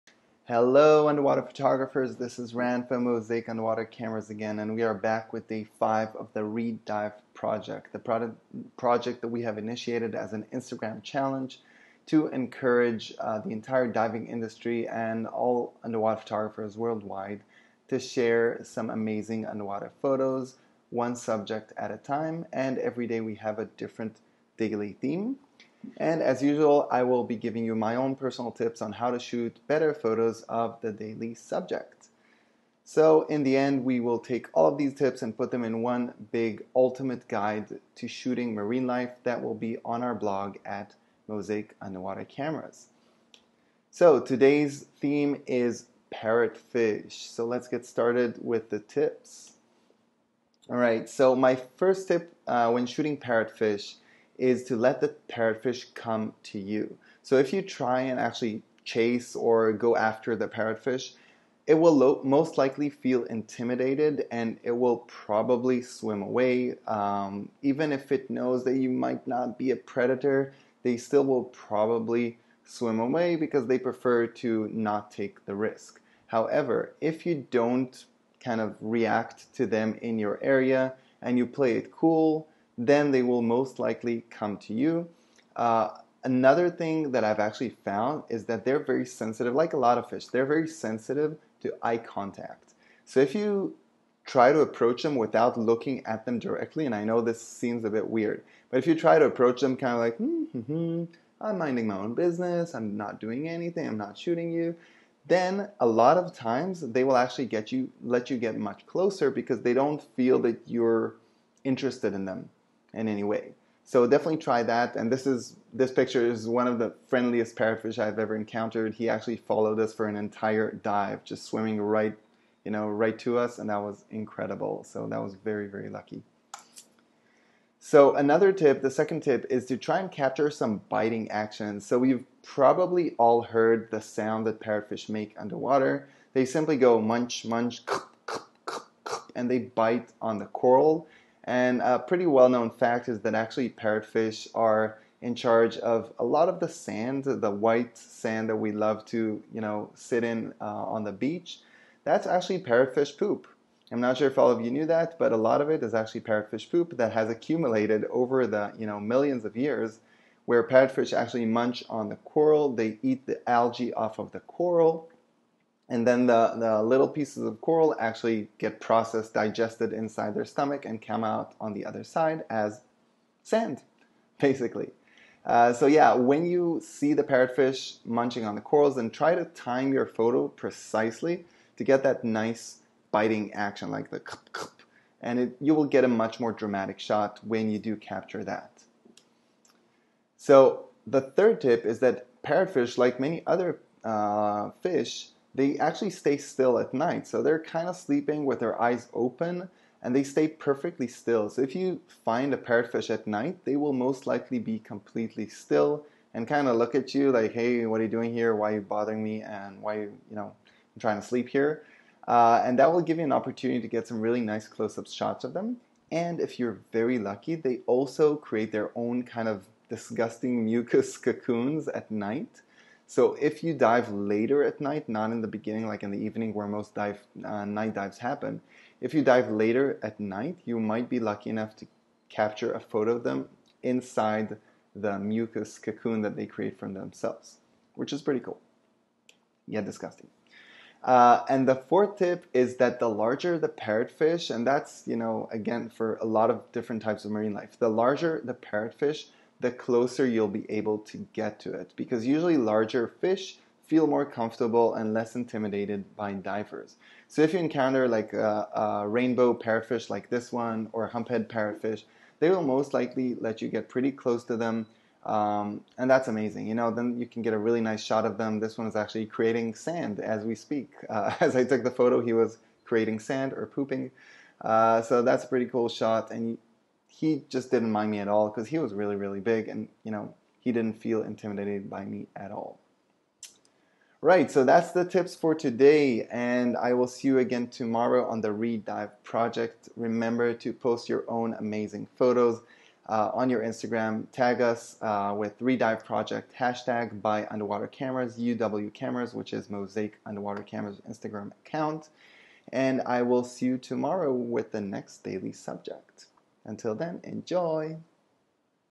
Tips for Shooting Parrotfish Underwater
Day #5 - Parrotfish!
As part of the ReDiveProject initiated by Mozaik UW Cameras, we are inviting underwater photographers from around the world to share their amazing photos, one subject at a time.

Here are some tips on how to shoot better photos of parrotfish!

Join the fun and share your photos! Tag them with the ReDiveProject hashtag :)